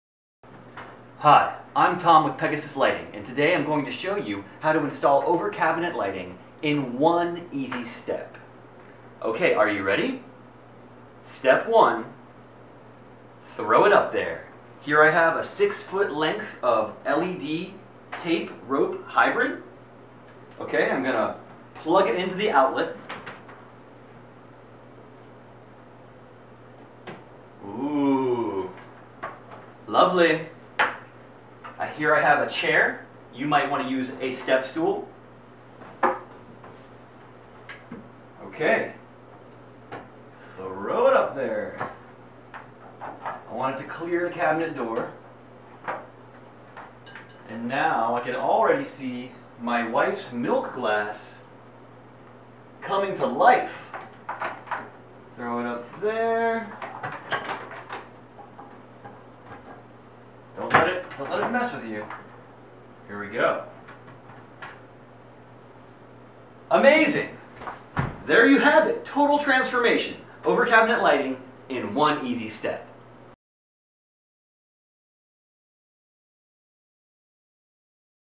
Apartment Lighting Project: Over Cabinet Lighting in One Easy Step
Tom from Pegasus Lighting improves the lighting in his rental's kitchen with LED over cabinet lights. In fact, it took him all of one minute to dramatically improve his kitchen using an LED tape/rope hybrid that can be found